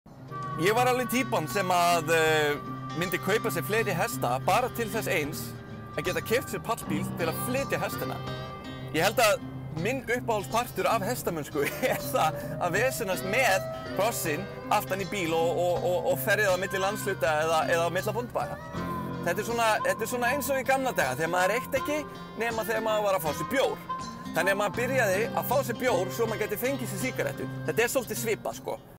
ISUZU D-max - Gerir hestamennskuna skemmtilegri
Hefur einhverjum dottið í huga að kaupa sér hesta og hestakerru… bara til að eiga afsökun til að kaupa sér pallbíl?